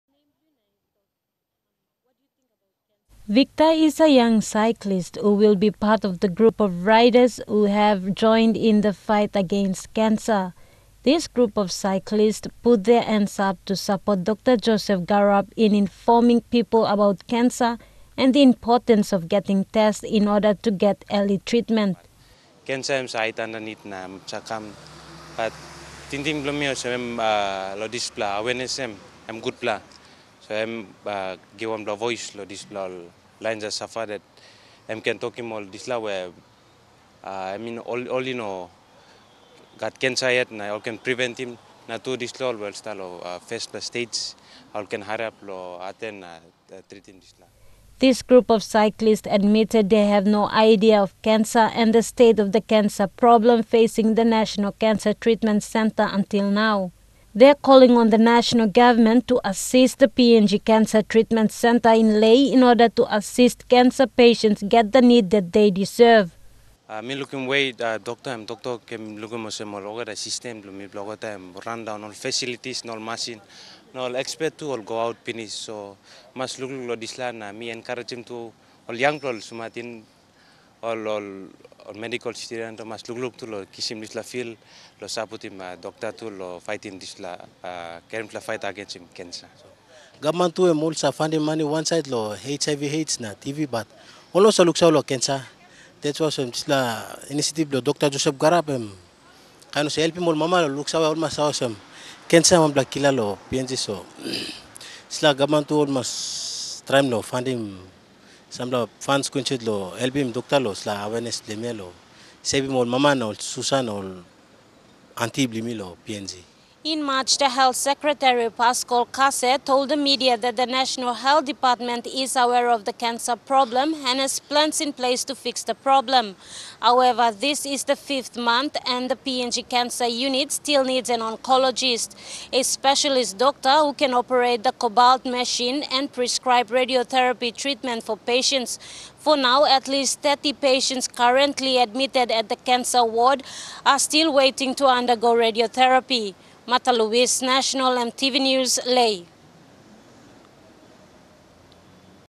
Lae Cyclist says More Cancer Awareness Needed
A cyclist in Lae says cancer is one of the diseases that need the same attention as the other illnesses have been getting from the national government.

Victor Apis, is one of the member of a group of cyclist who are going around in residential areas informing families about the signs and symptoms of cancer.